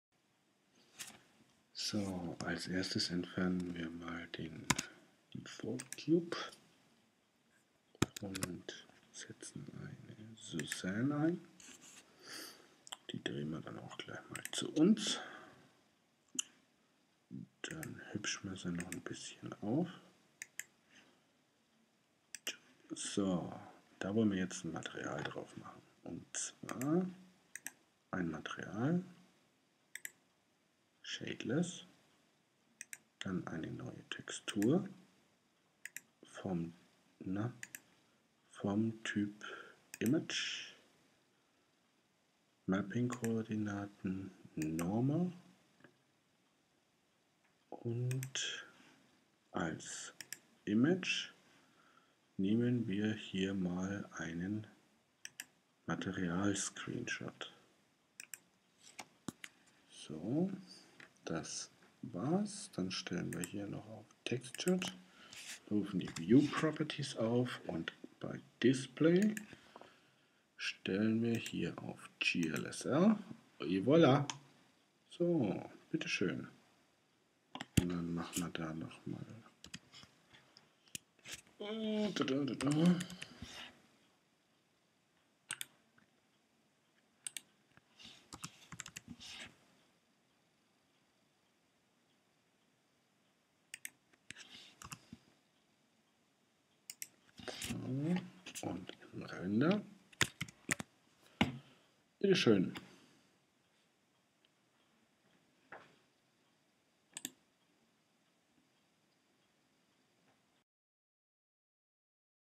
Texturieren in Blender mit MatCaps
Verwenden von Material Captures (MatCaps - Screenshots) zum schnellen Rendern z.B. von spiegelnden Oberflächen.